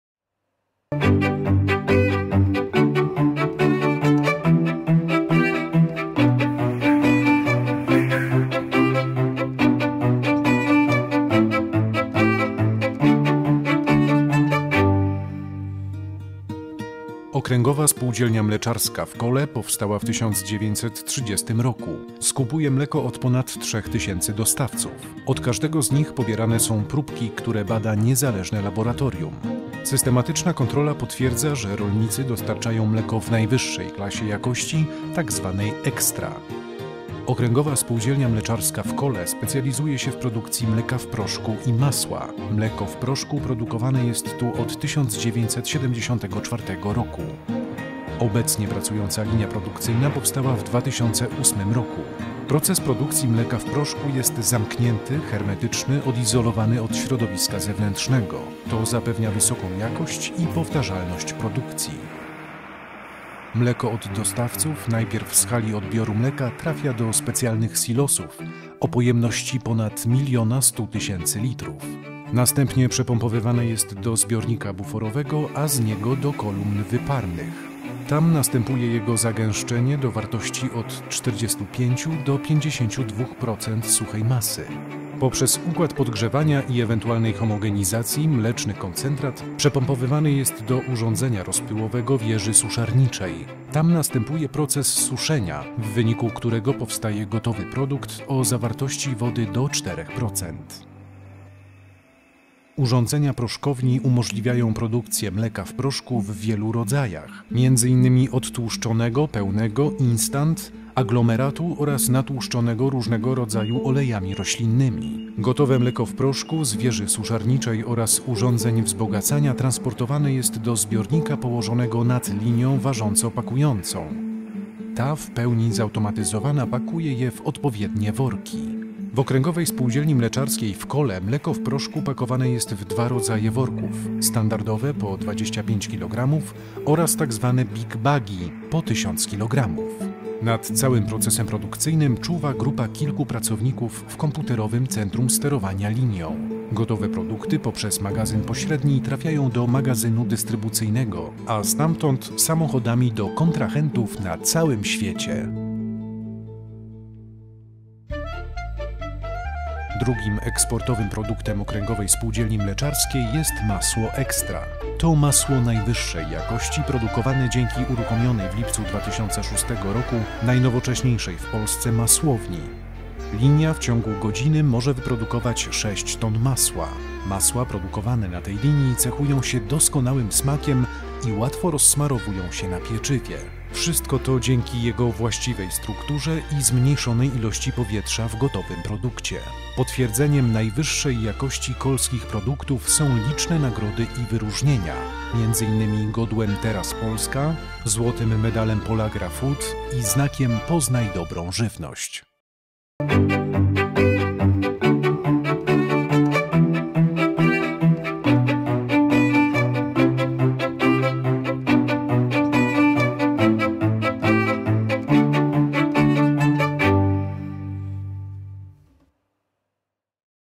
Film mleko,masłoPL
OSM w Kole, proces produkcji mleka w proszku oraz masła.